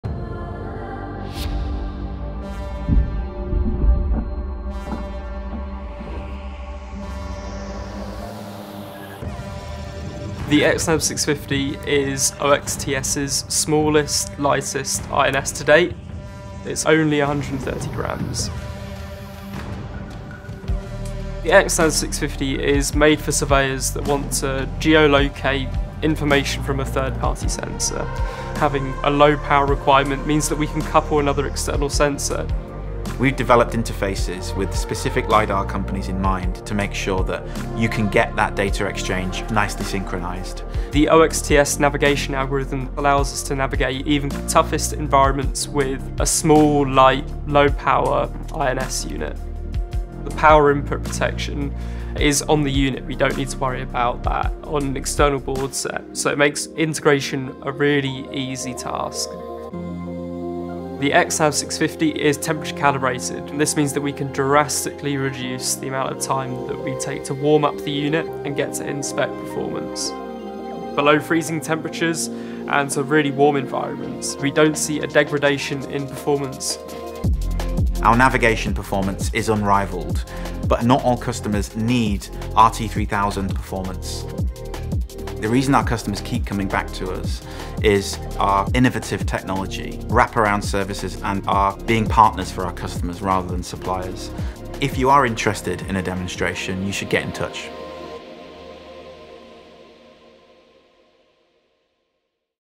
xNAV650 - when accuracy and reliability with a small footprint is needed
The xNAV650 INS is one of our smallest, lightest and most affordable Inertial Navigation Systems.

Weighing just 130 g and measuring only 77 x 64 x 24 mm the xNAV650 is the ideal companion for applications where size and weight are important considerations to make.

With free post-processing software, and optional LiDAR georeferencing and boresight calibration tools available, the accurate, reliable xNAV650 is made for survey and mapping applications.